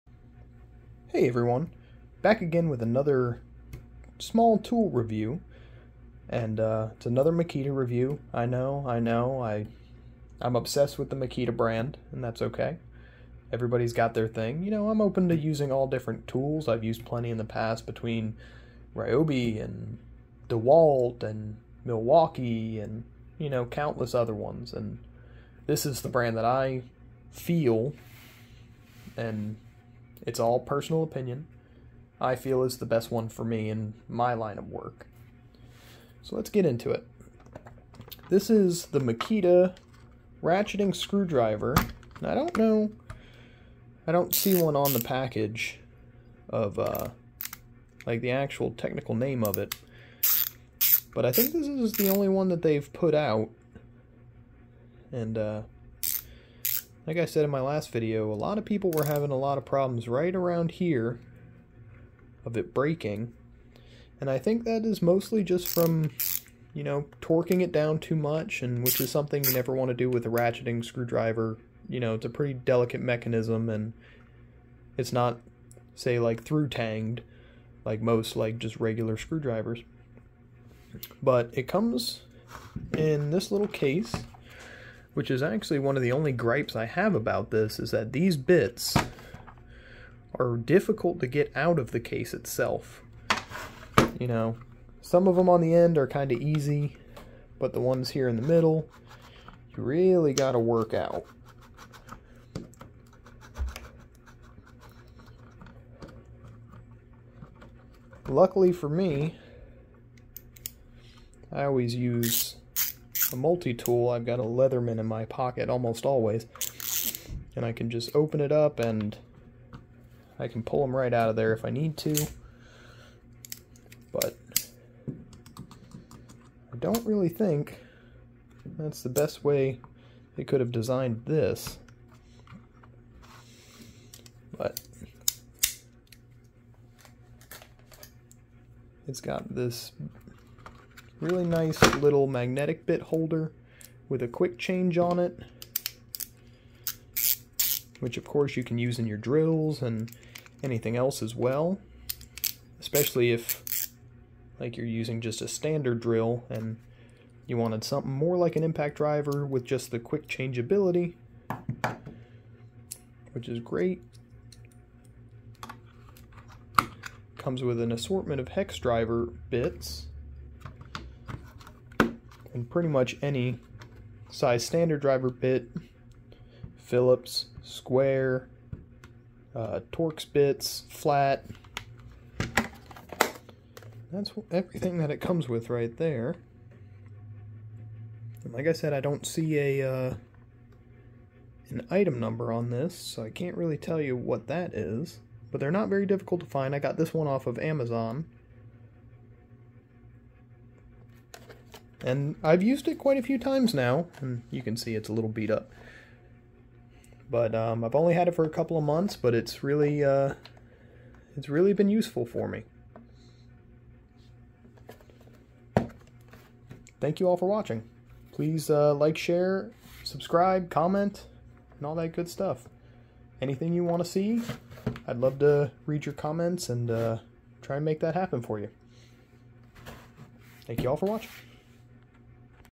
Makita B-50289 Review
Makita ratcheting screwdriver impressions and review. A great tool for taking off wallplates when painting and the simple jobs that don't require a ton of torque.